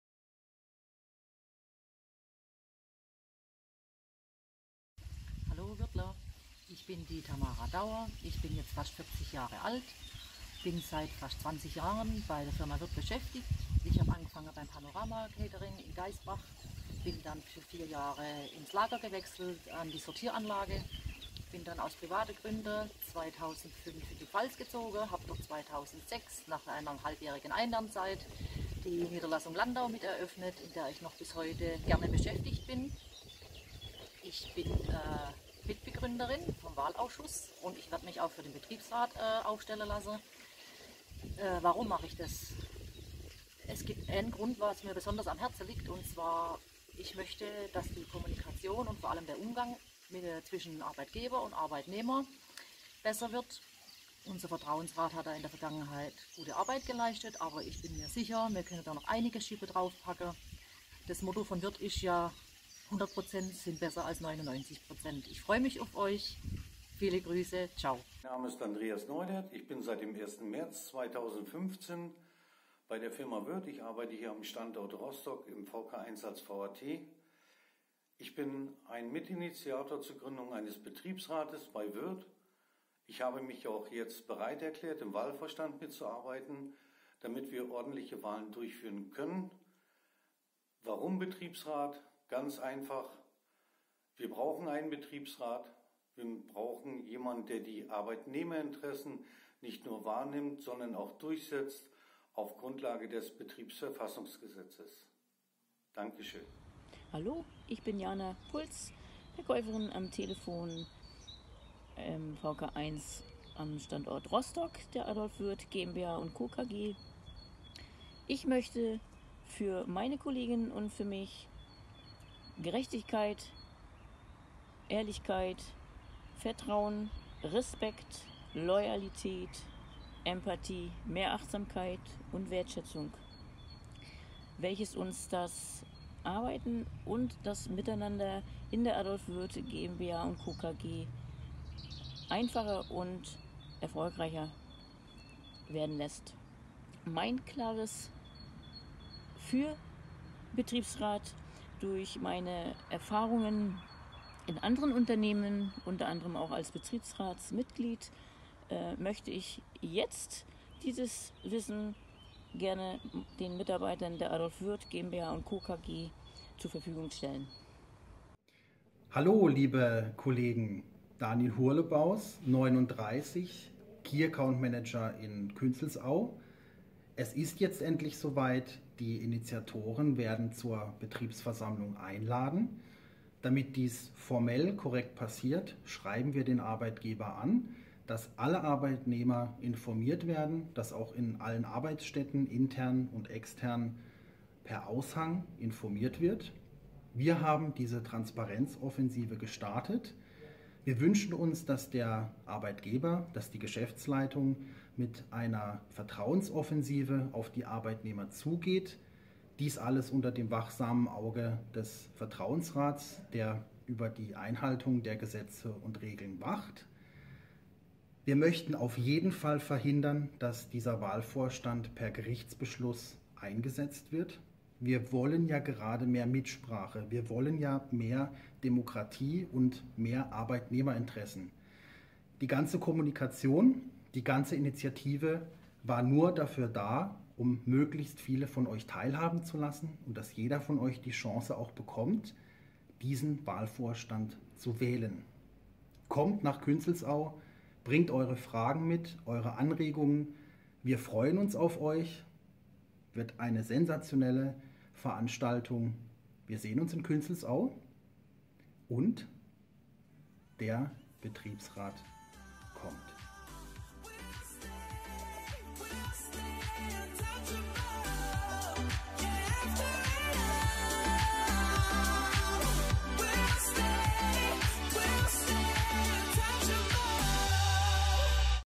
DIE INITIATIVE ZEIGT GESICHT
Die ersten 4 mutigen Initiatoren stoßen nun den formellen Prozess nach dem Betriebs-Verfassungsgesetz zur Wahl eines Betriebsrats an:

Tamara Dauer

Jana Puls

Andreas Neudert

Daniel Hurlebaus